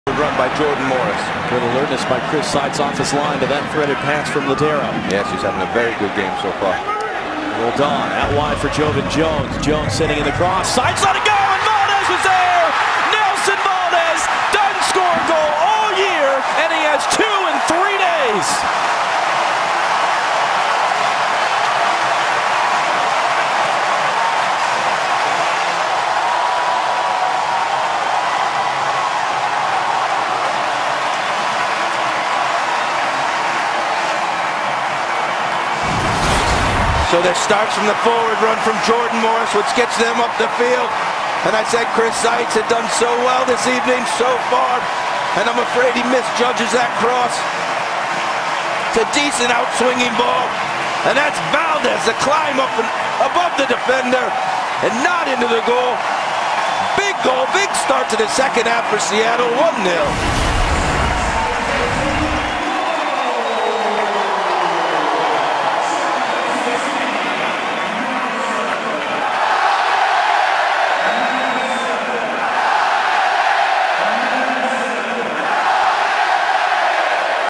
GOAL: Nelson Valdez opens the scoring with his second of the playoffs
Seattle Sounders FC forward Nelson Valdez scores his second goal of the playoffs in the 50th minute vs FC Dallas.